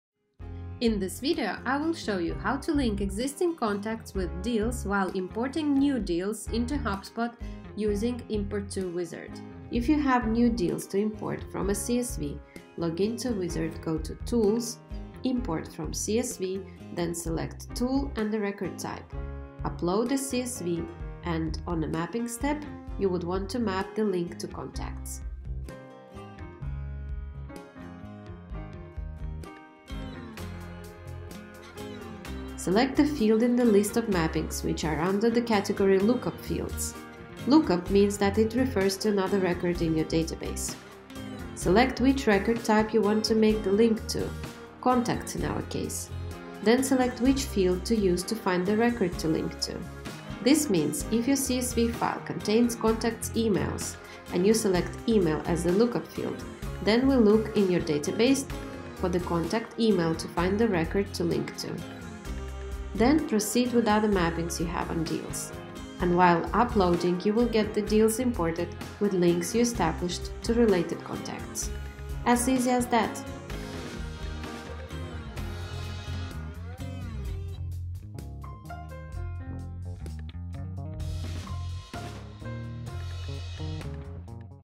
Import Deals into HubSpot (and Link them to Contacts)
This video shows how to link existing contacts with deals while importing new deals into HubSpot.